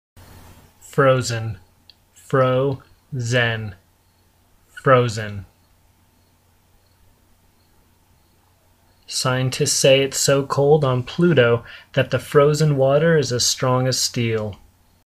How to pronounce Frozen
Learn how to pronounce Frozen and use in a sentence.